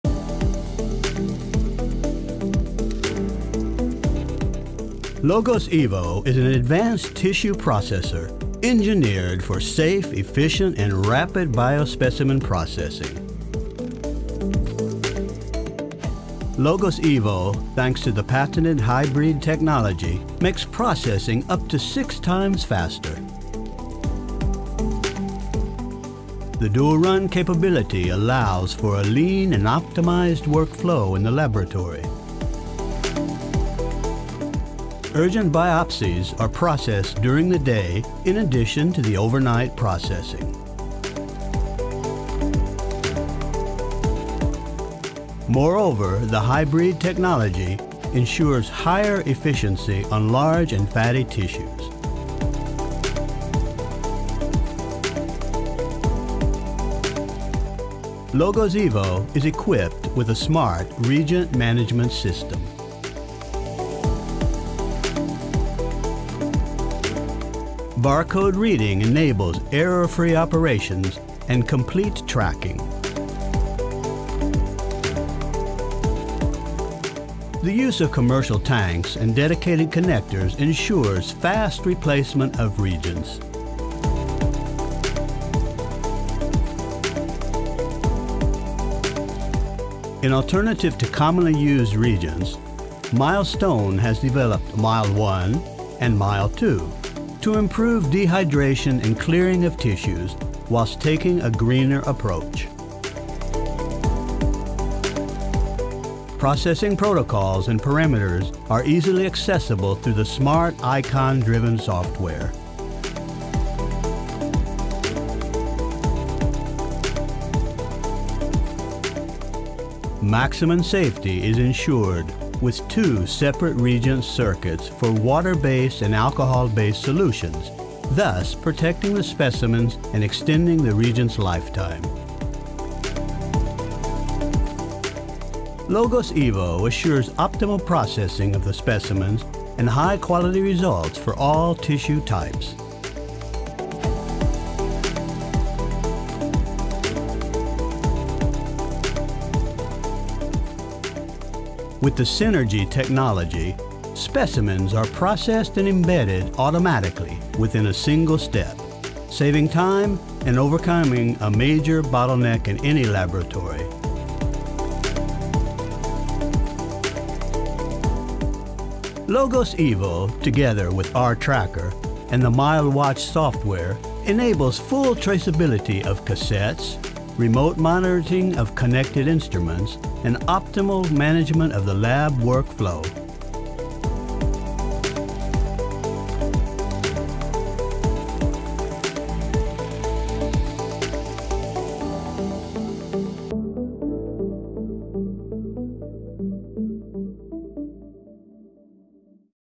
LOGOS EVO - Better Workflow, Enhanced Safety - extended version
Conventional processing of biospecimen relies on an outdated technology that has inherent limitations in terms of both process time and workflow. Milestone is committed to offer innovative and practical solutions to overcome the well-known challenges of the crucial pre-analytical phase. LOGOS EVO tissue processor uses advanced technology, software, and reagents to enhance productivity and safety - combined with a green approach. LOGOS EVO offers new features for a faster and even more reliable processing.

- OPTIMIZED WORKFLOW
- SMART REAGENT MANAGEMENT
- SAFER FOR BOTH OPERATORS AND SPECIMENS
- INTUITIVE USER INTERFACE
- ENHANCED TRACEABILITY